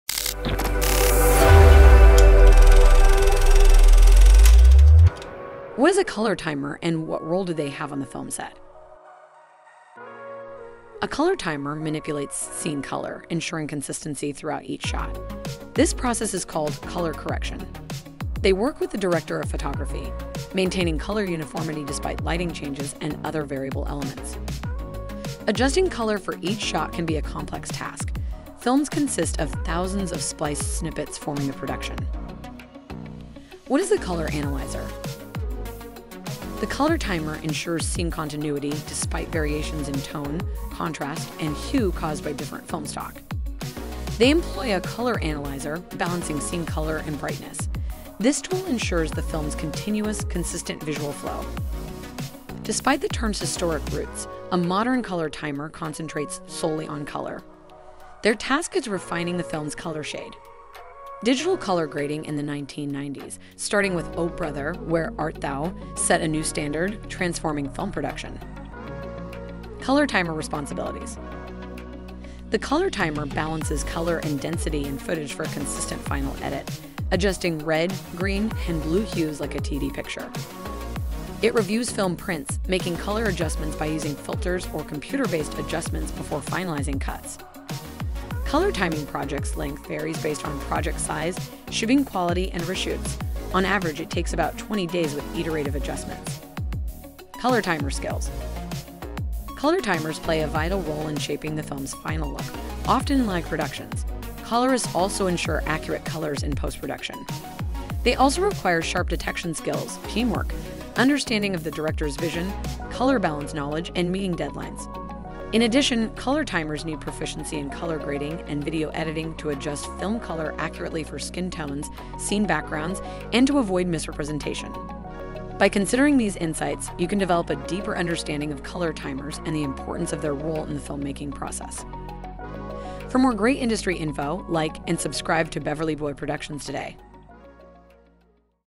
WHAT IS A COLOR TIMER & WHAT ROLE DO THEY HAVE ON THE FILM SET?
Discover the role of a "Color Timer" on the film set with our informative video. Get an insider's look into the world of filmmaking and learn about the art and science of color correction, grading, and the use of advanced tools like the color analyzer.

This video breaks down the complex responsibilities of a color timer, explaining how they work meticulously with the Director of Photography to ensure the perfect balance of color, hue, and saturation. We dive into the evolution of color timing, from traditional film splicing to today's digital color grading.

Learn how films like O Brother, Where Art Thou? revolutionized the industry, and explore how color timers bring a director's vision to life by enhancing visual storytelling. By understanding color timer skills and the challenges they face, you'll gain a new appreciation for this crucial role in the film industry.

0:00 WHAT IS A COLOR TIMER & WHAT ROLE DO THEY HAVE ON THE FILM SET?
0:32 WHAT IS THE COLOR ANALYZER?
1:09 COLOR TIMER RESPONSIBILITIES
1:37 COLOR TIMER SKILLS
2:12 SUBSCRIBE TO OUR CHANNEL

Whether you're an aspiring filmmaker or a cinema enthusiast, this video will provide a fresh perspective on a less-known but fundamental aspect of movie production.